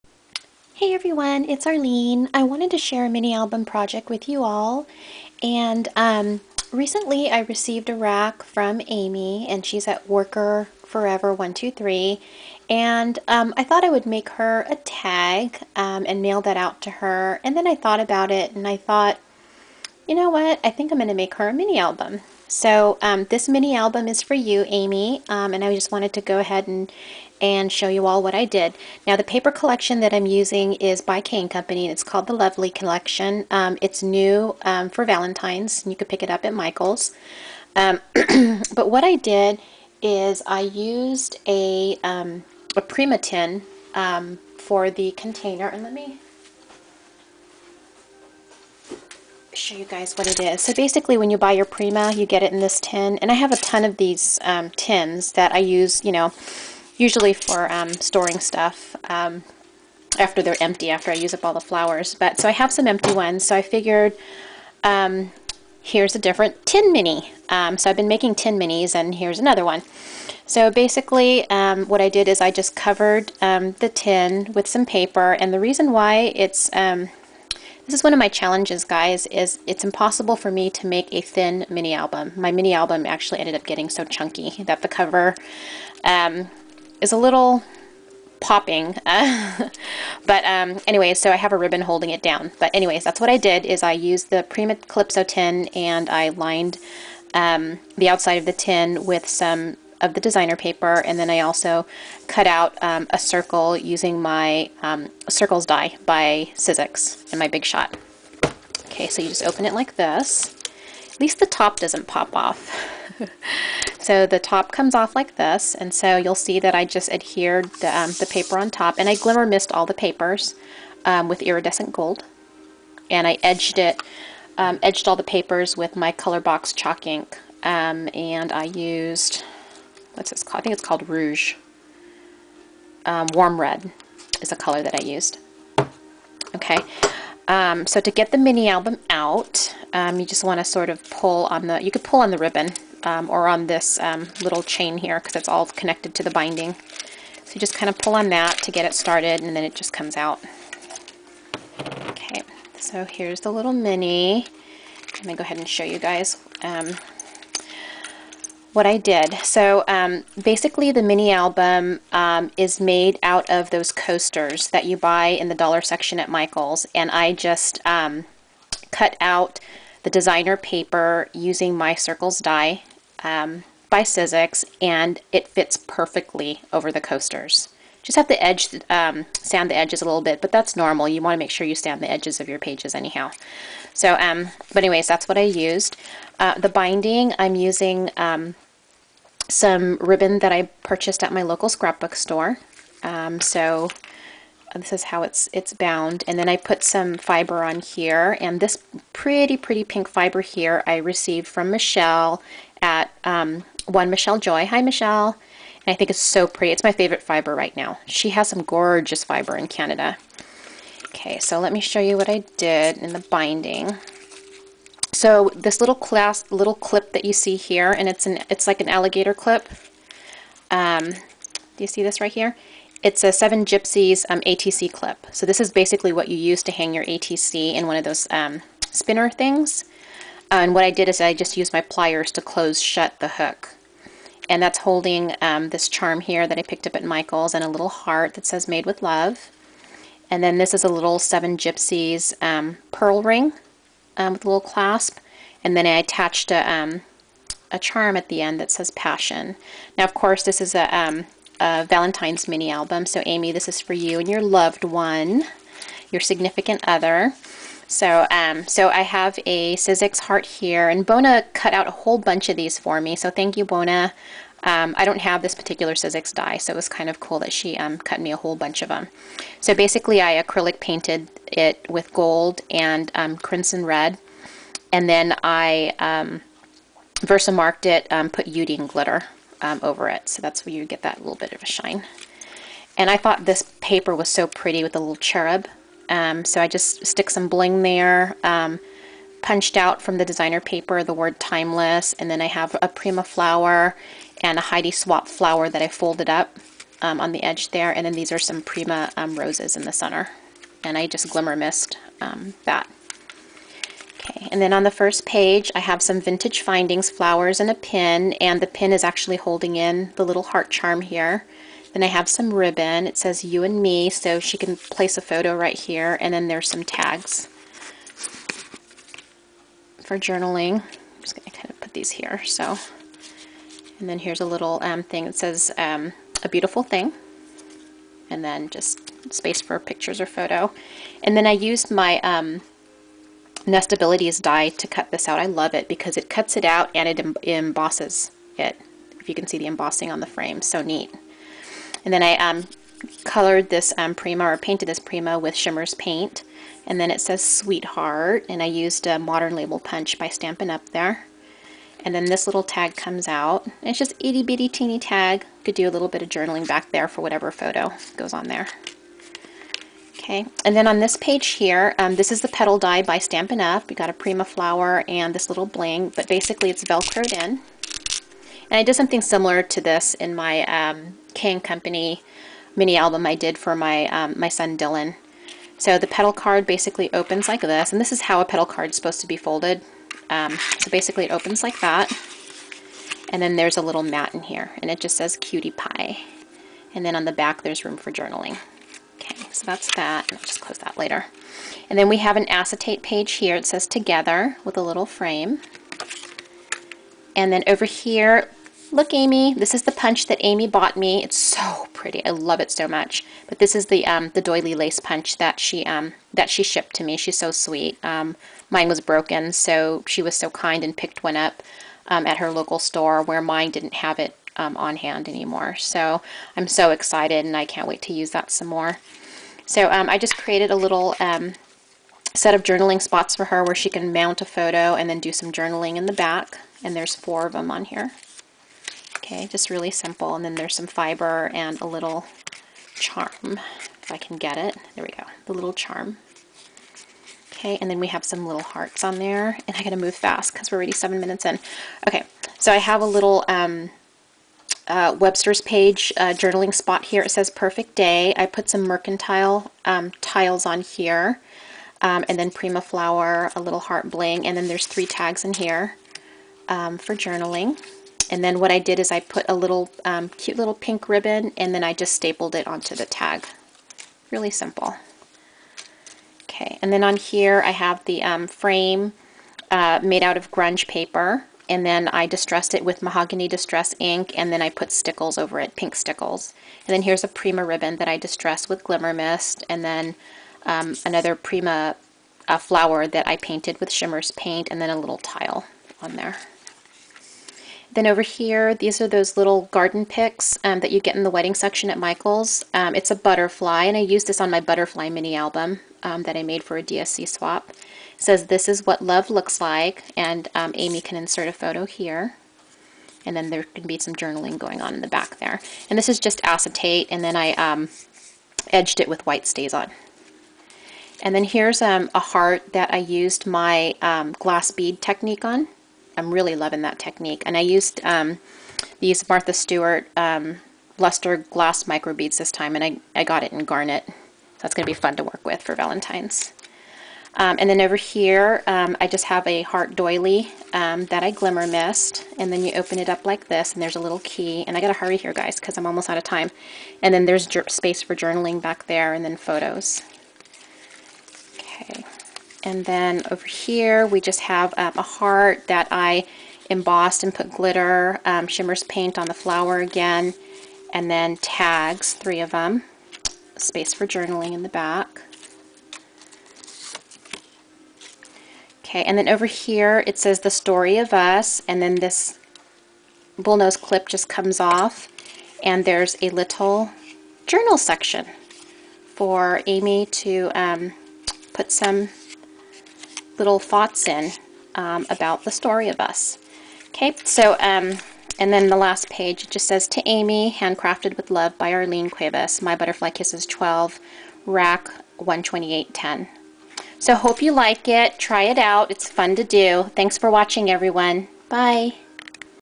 Valentine's Mini Album Tin for Amy!
Hi ladies. I created this Valentine's mini album tin for Amy to say Thank You for her kindness, generosity and for being such a sweetheart! :-) I had a ton of fun making this tin mini and I hope you give it a try. :-)